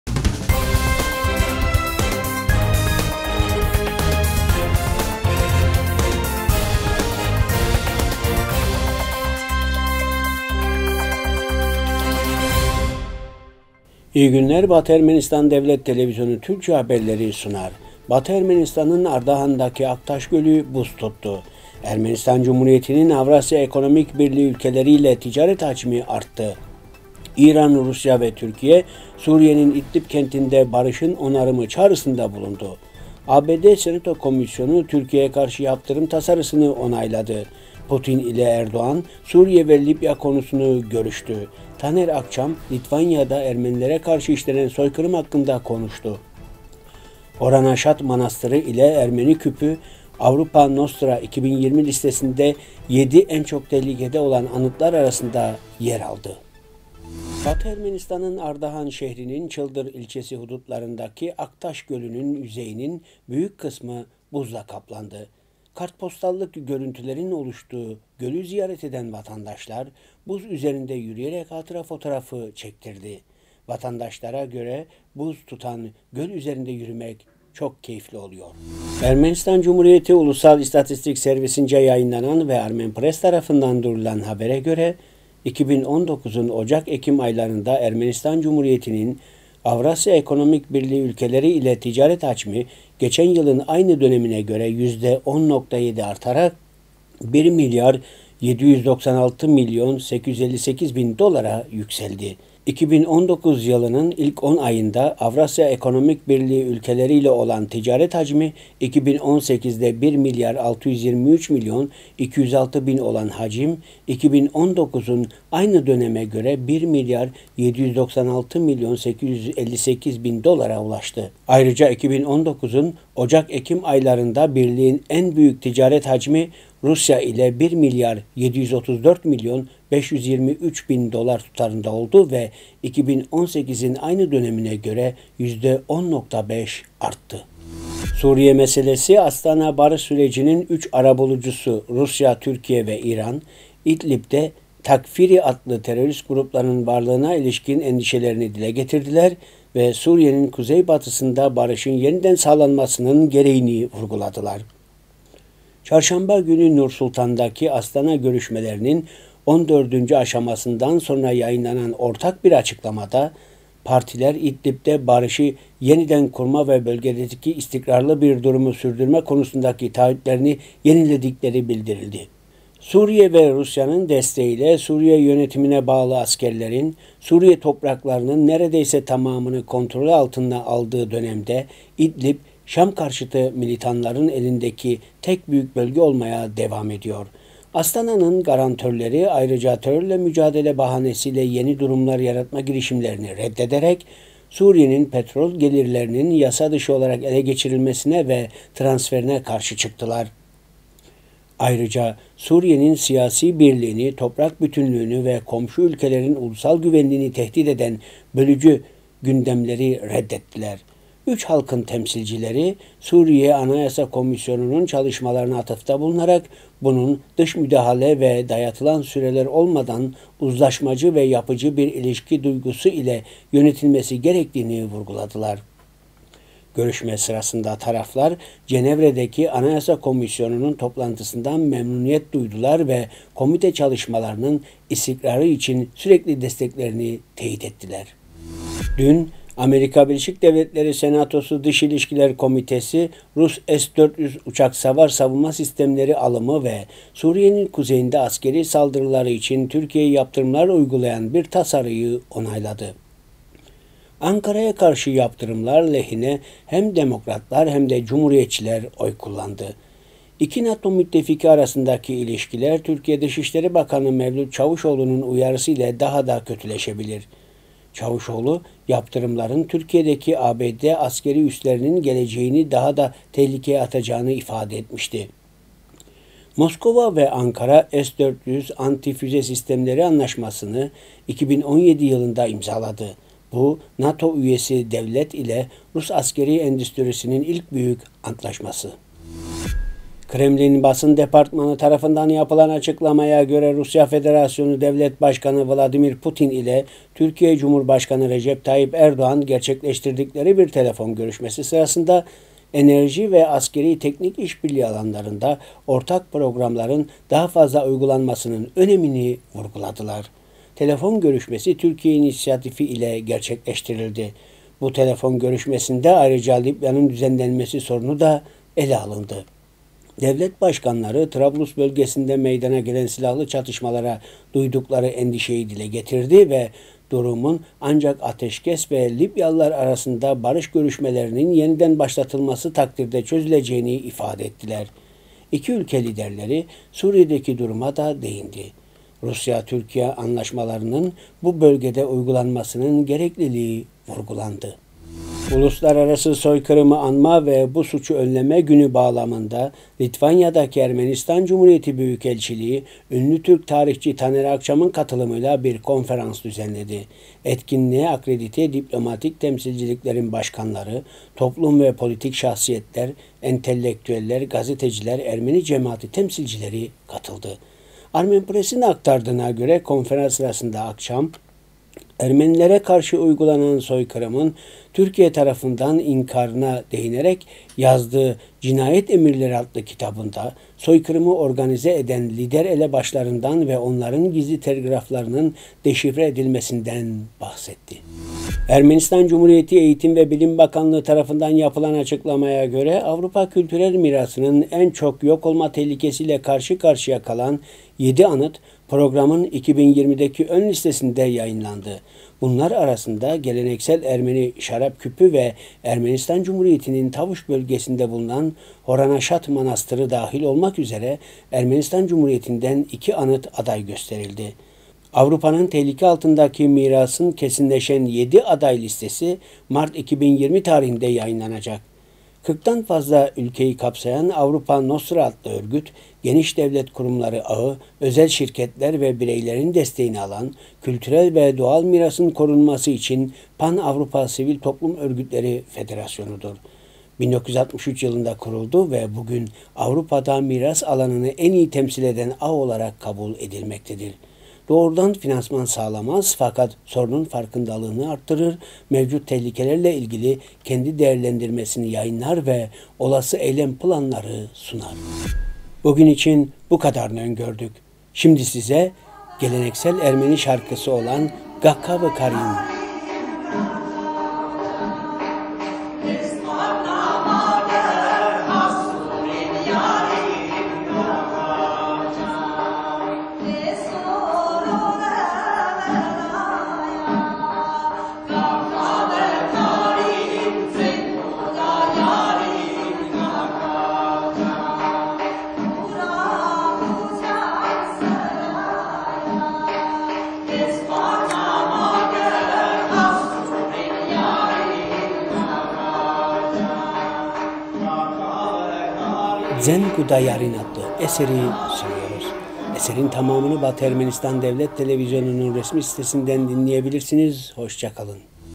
Taner Akçam, Litvanya'da Ermenilere karşı işlenen soykırım hakkında konuştu .Haberler 12-12-2019
1- Batı Ermenistan’ın Ardahan’daki  Aktaş Gölü buz tuttu
2- Ermenistan Cumhuriyeti’nin Avrasya Ekonomik Birliği ülkeleriyle ticaret hacmi arttı
3- İran, Rusya ve Türkiye, Suriye’nin İdlib kentinde barışın onarımı çağrısında bulundu
4- ABD Senato Komisyonu, Türkiye'ye Karşı Yaptırım tasarısını onayladı
5- Putin ile Erdoğan, Suriye ve Libya konusunu görüştü
6- Taner Akçam, Litvanya'da Ermenilere karşı işlenen soykırım hakkında konuştu
7- Khoranaşat manastırı ile Ermeni küpü, “Avrupa Nostra” 2020 listesinde 7 en çok tehlikede olan anıtlar arasında yer aldı